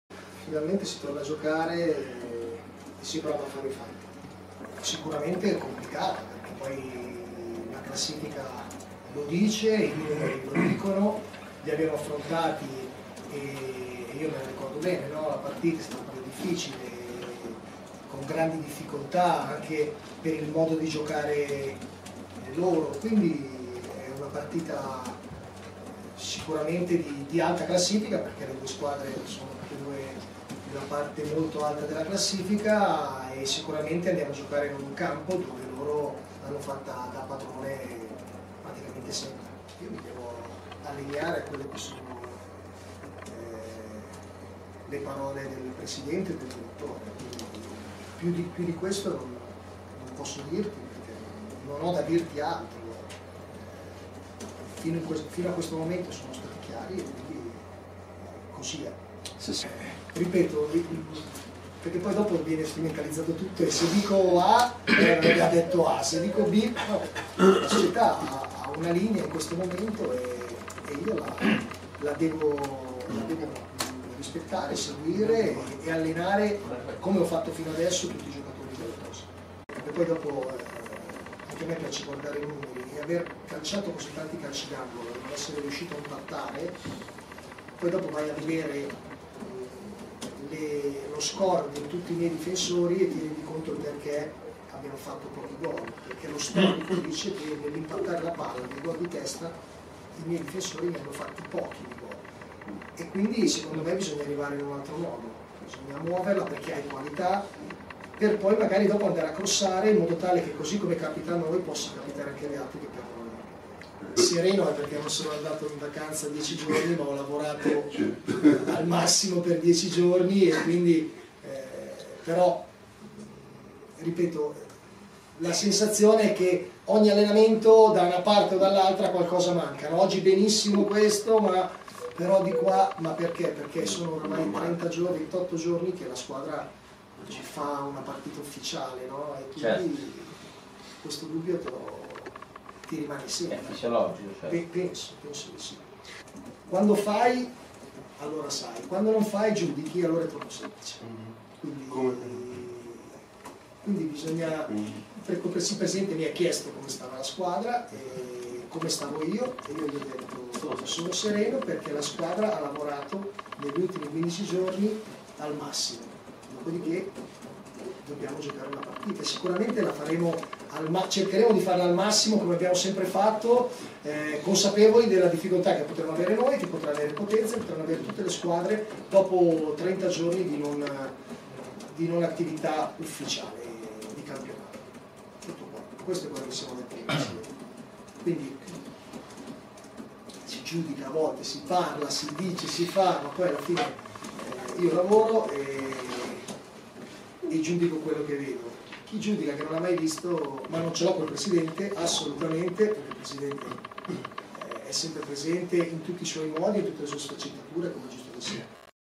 Ternana, Gallo pre Potenza: «Mercato? Devo rispettare linea societaria. Match complicato»
Le parole del tecnico rossoverde alla vigilia del confronto con il Potenza: spazio al confronto con il presidente Stefano Bandecchi